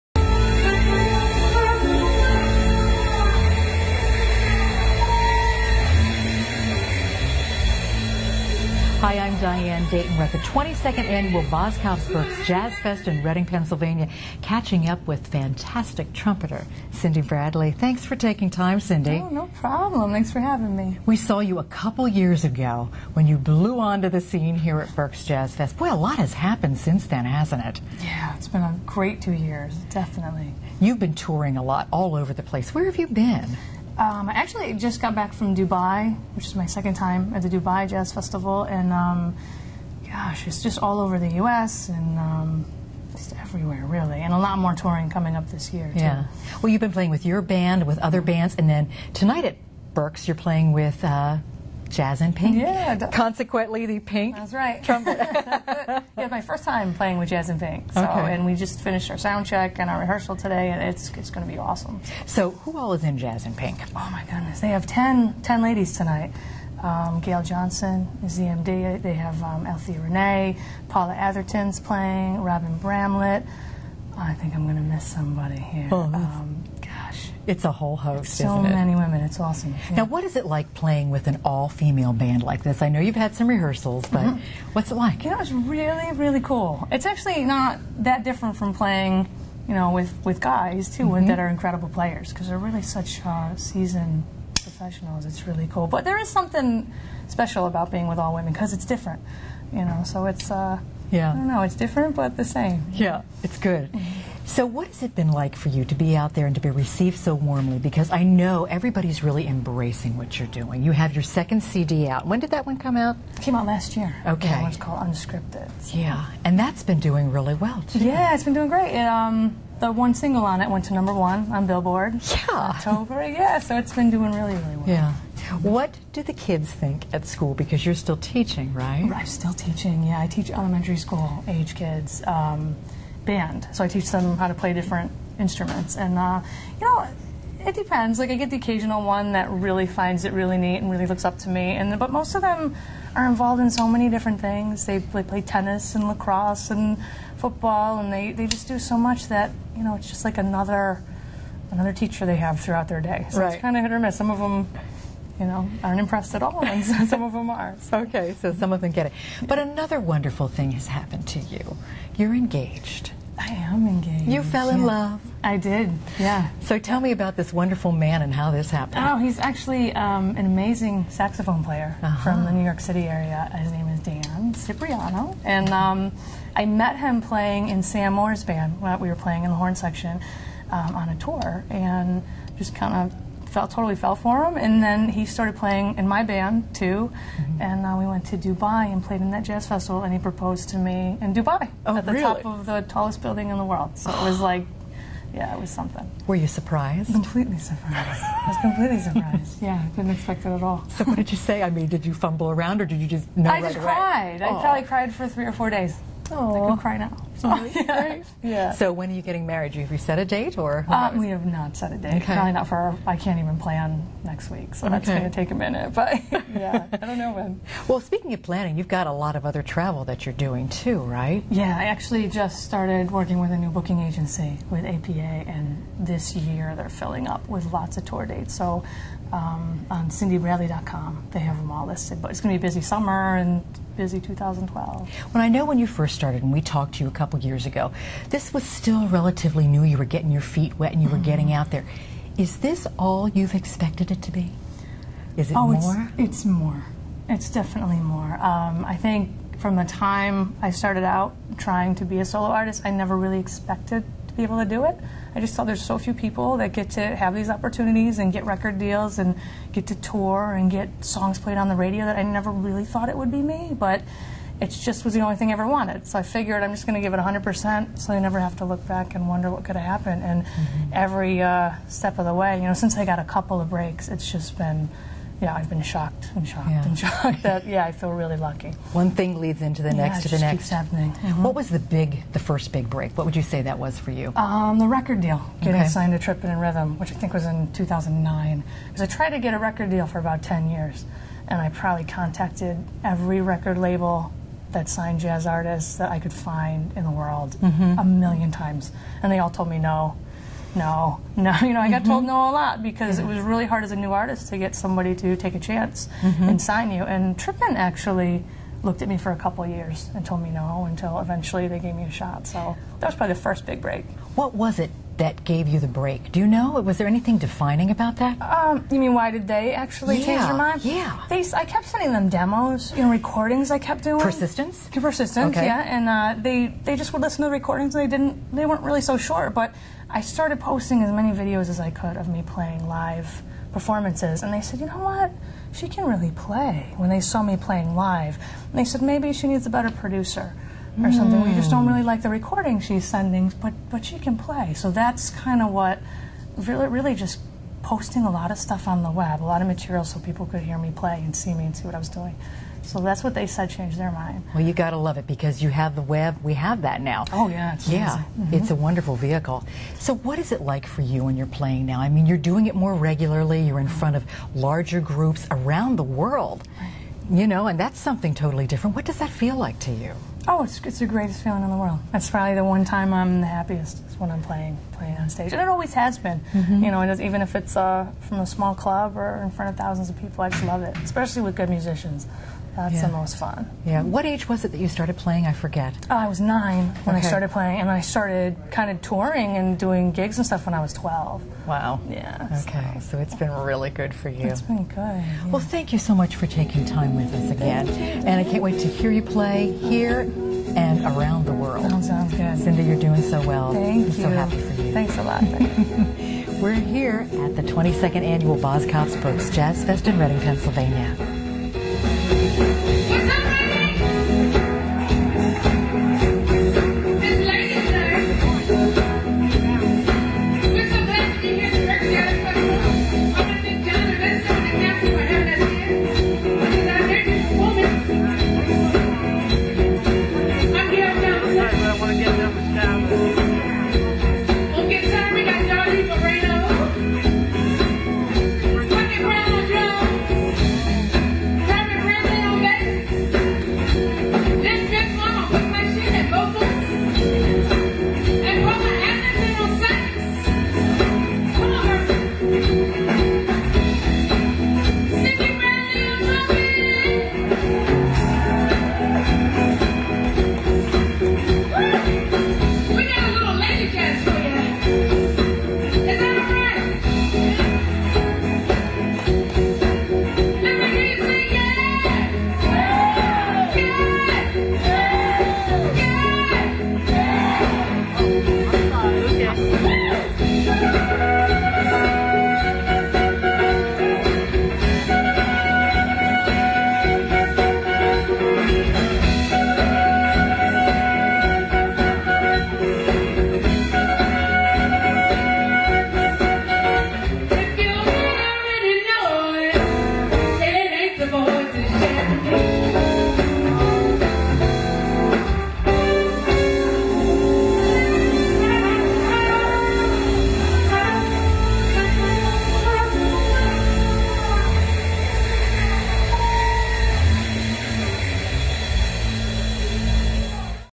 Cindy Bradley 2012
Trumpeter, Cindy Bradley catches up with Diane Dayton of Cool Jazz Café at Berks Jazz Fest 2012.  Cindy shares about the journey & defining moment to get a record deal, her engagement and the proposal and playing with the female band at Berks "Jazz in Pink." A short clip with Gail Jhonson and Jazz in Pink included.